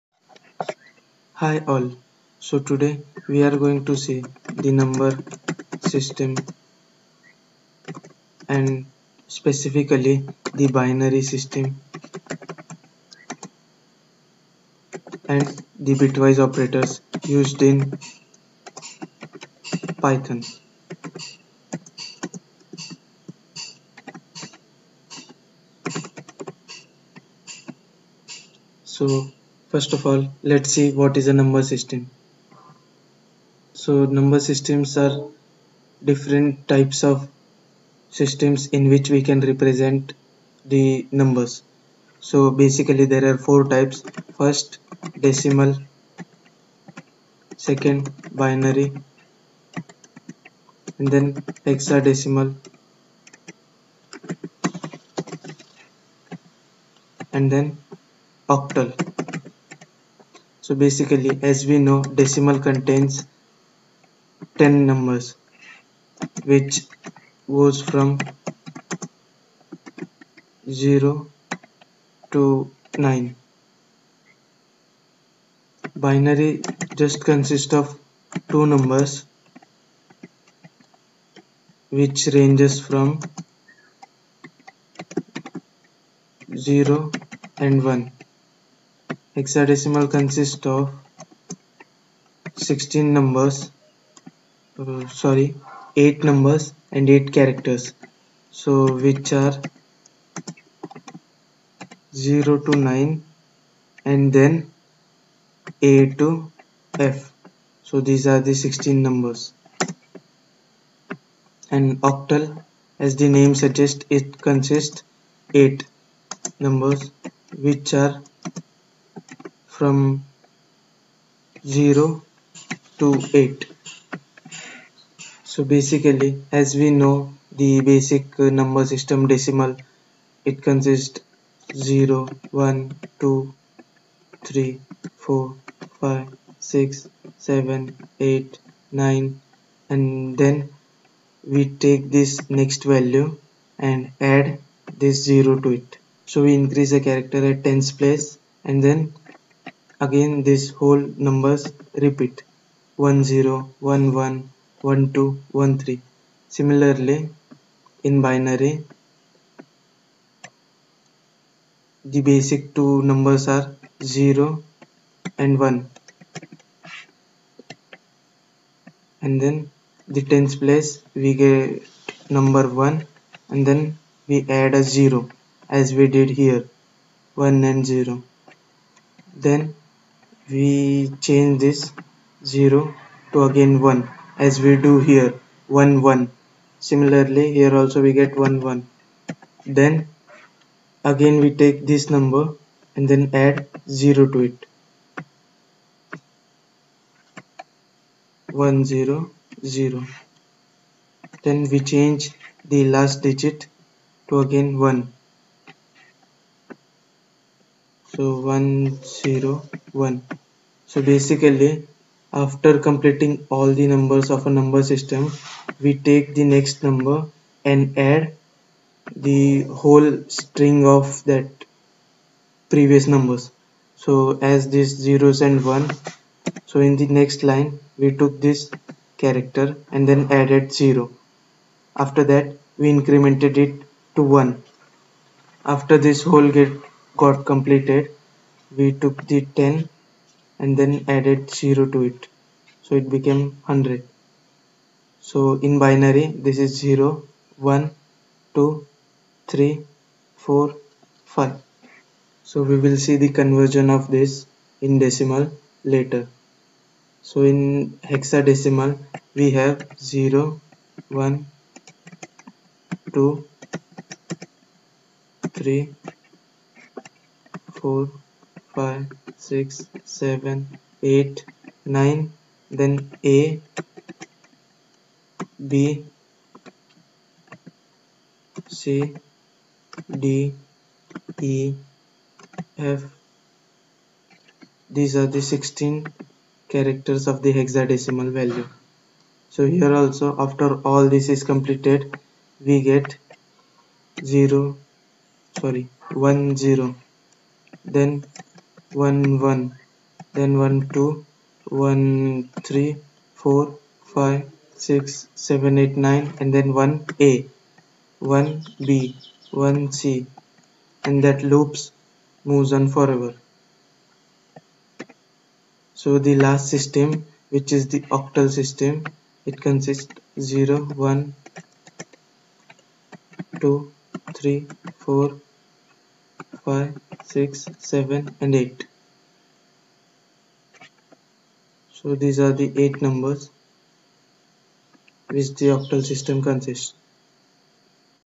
Number Systems-22 | Learning Language 19 | GOLD-IN-ICPC-FOR-INDIA | VIDEO SERIES
These are some Perquisite for the Bitwise Operators Video.
This video Explains you the Various Number System Out There are More Specifically The Binary System.
This is Part 1 of series of video on Binary and Bitwise Operators.

Wanna Ask Some Doubts??